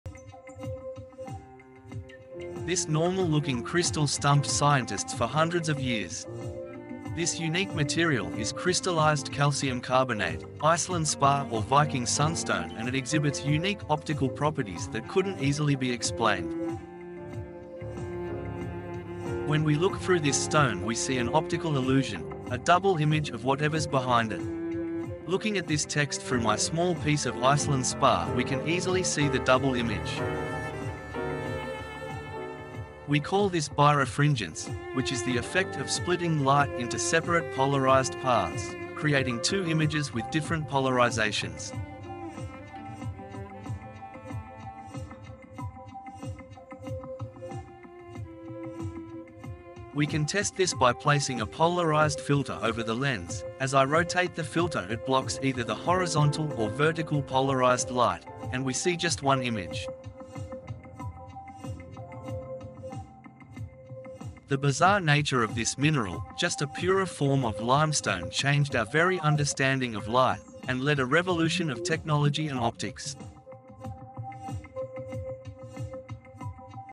Iceland Spar. The stone that changed our understanding of light.
This unique material is Crystallised calcium carbonate, Iceland Spar or Viking Sunstone and it exhibits unique optical properties that couldn't easily be explained